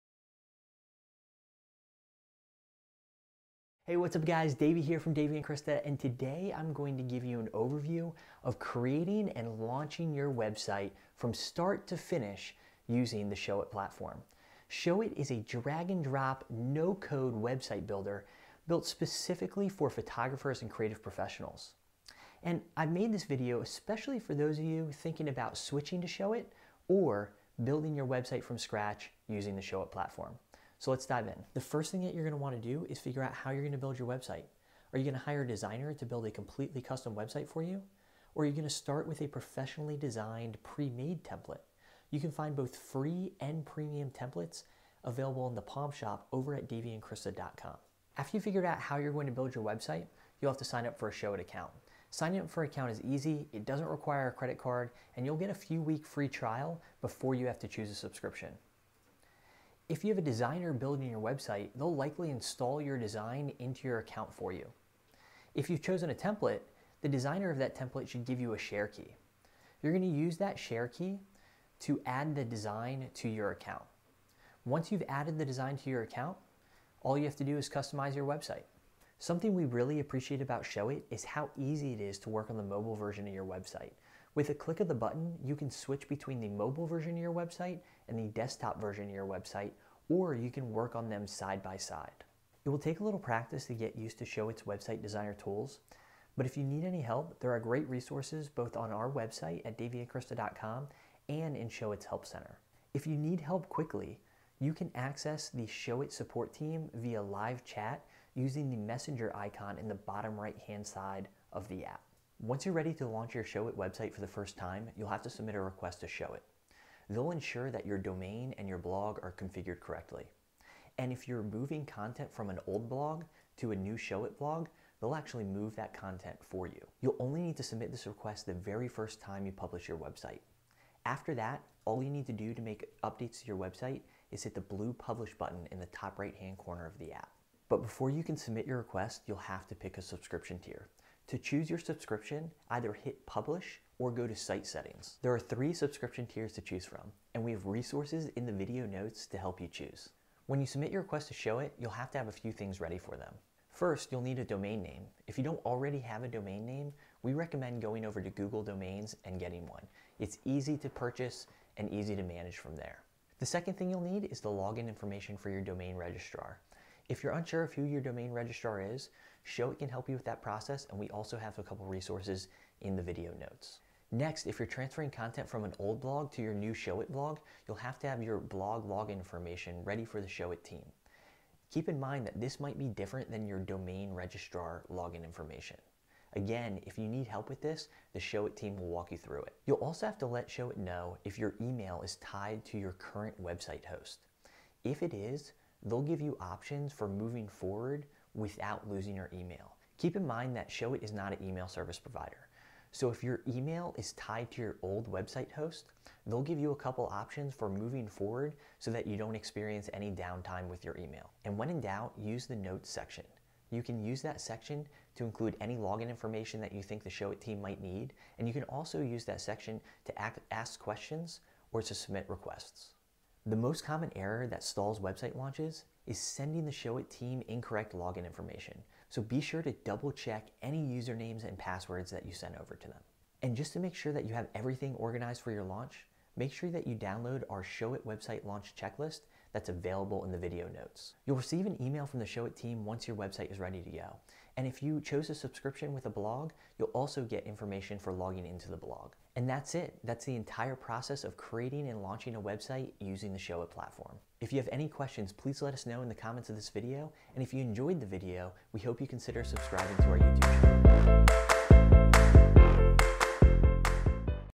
An Overview of Creating and Launching a Showit Website
Showit is a no code, drag-and-drop website builder for photographers and creative professionals. This less than 5 minute overview explains how to create and launch a Showit website from start to finish.